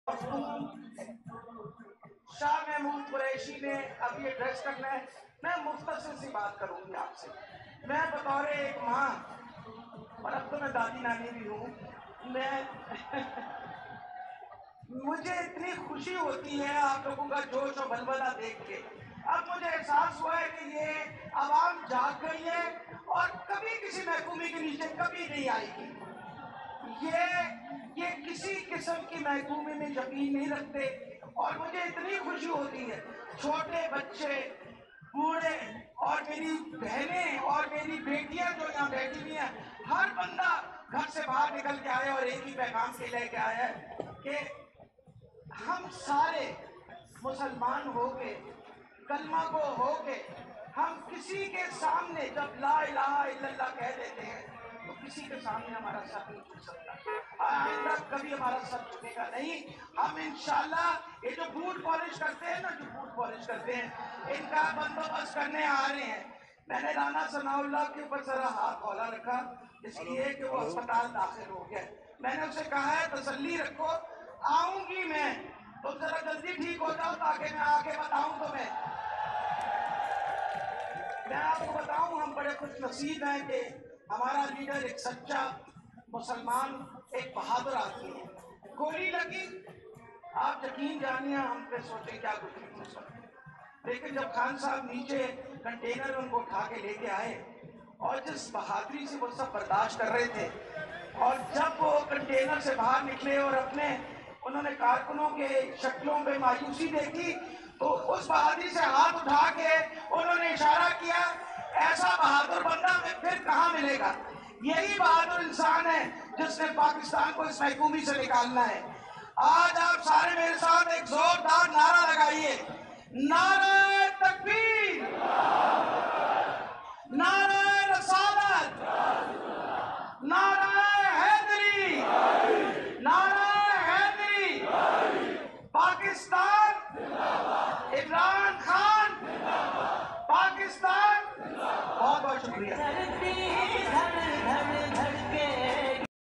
Sarai Alamgir Punjab, Dr. Yasmeen Rashid's touching speech at the PTI Long March
سرائے عالمگیر پنجاب ‘ ڈاکٹر یاسمین راشد کا پی ٹی آئی لانگ مارچ سے ولولہ انگیز خطاب

Welcome to the Official Youtube Channel of  Qudrat Tv ,